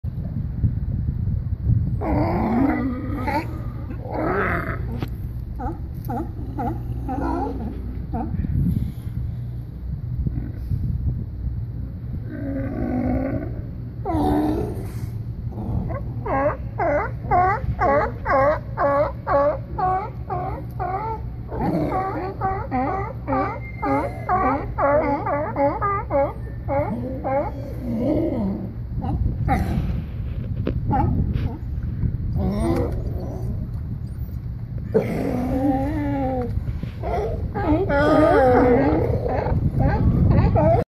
Westport Sea Lions 2/10/2018
The barking of sea lions fills the air at the waterfront in Westport,  Washington.